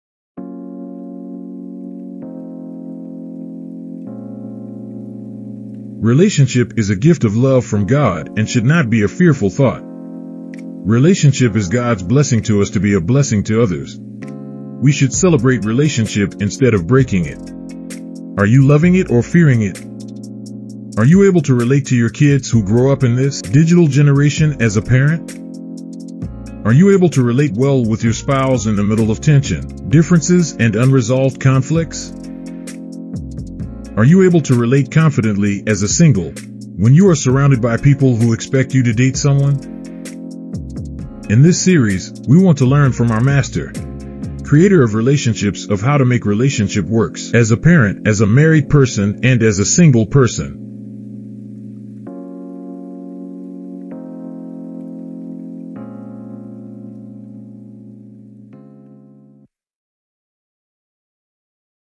SBC ENGLISH SERVICE SERIES - 𝙍𝙀𝙇𝘼𝙏𝙄𝙊𝙉𝙎𝙃𝙄𝙋𝙎 𝙄'𝙈 𝙇𝙊𝙑𝙄𝙉 𝙄𝙏
𝗘𝗻𝗴𝗹𝗶𝘀𝗵 𝗦𝗲𝗿𝘃𝗶𝗰𝗲 | Sunday 8:00am - 9:30am

Relationship is a gift of love from God and should not be a fearful thought. Relationship is God's blessing to us to be a blessing to others. In this series, we want to learn from our Master, Creator of Relationships of how to make relationship works as a parent, as a married person and as a single person.